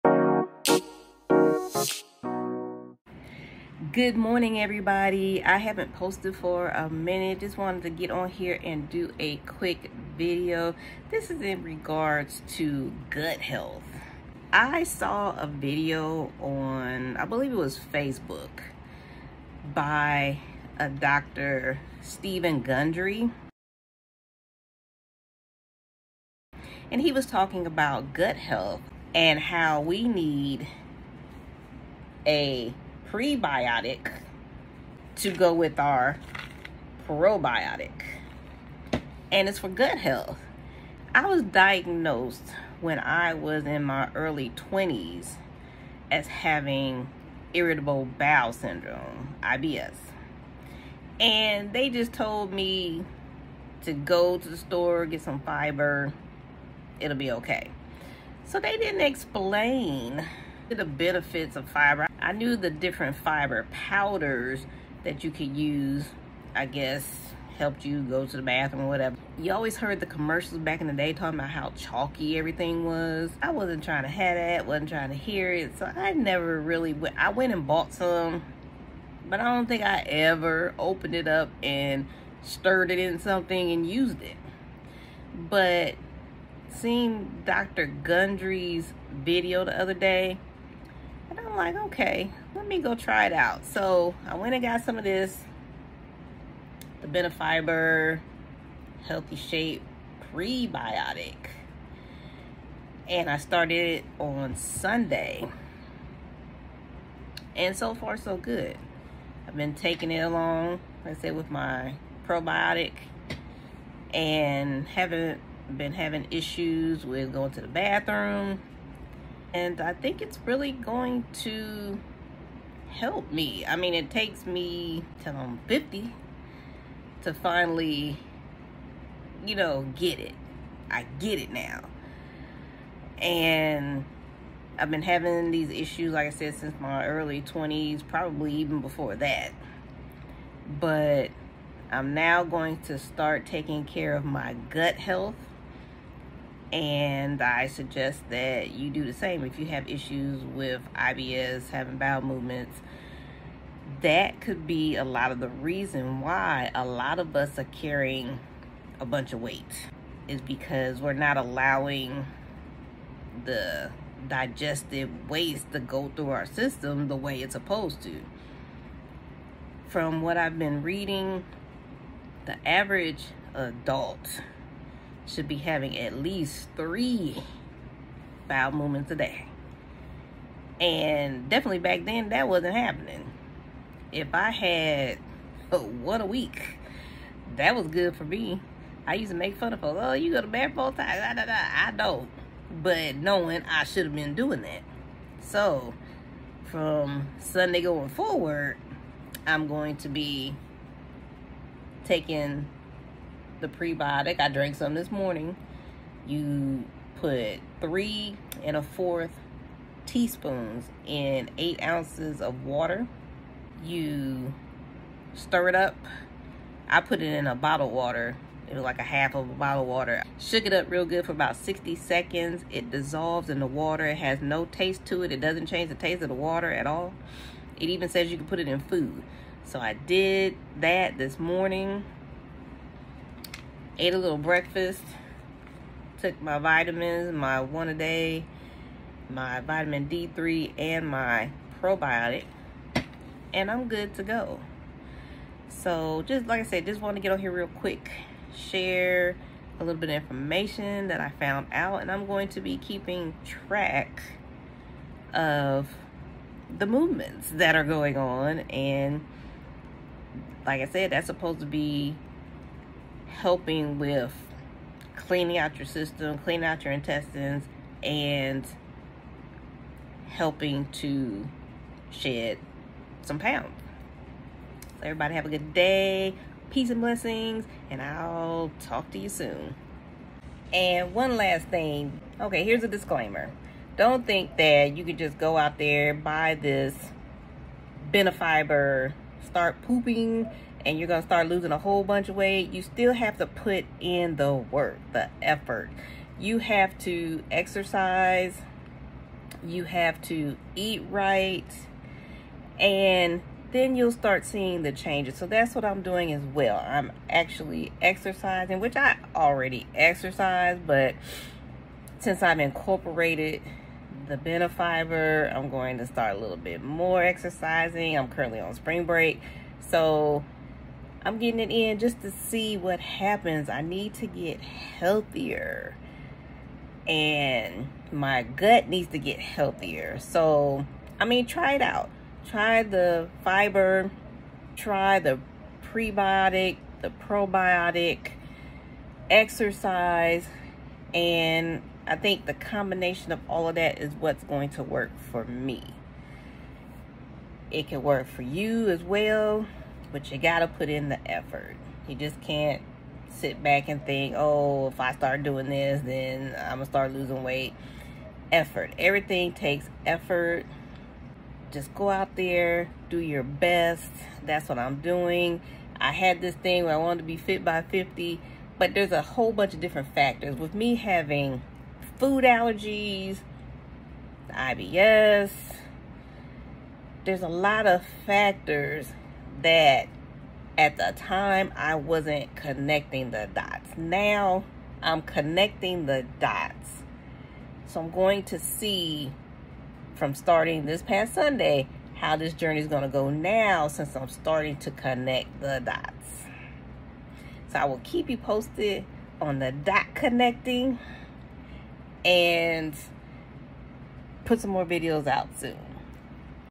Gut health discussion
I Finally Get It! I talk about being diagnosed with IBS in my 20’s but just now connecting the dots as to what I need to do for good gut health. This is all thanks to a video I watched by Dr. Steven Gundry.